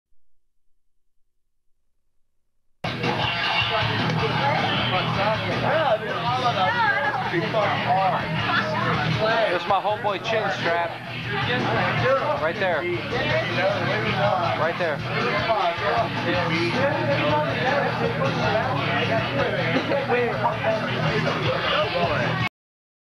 Chin Strap Sighting - Chris Gianella
Here is a snipet of the infamous "Chin Strap" Chris Gianella (RIP). Captured from our 20 Dead Flower Children Tour video (HB) 1997.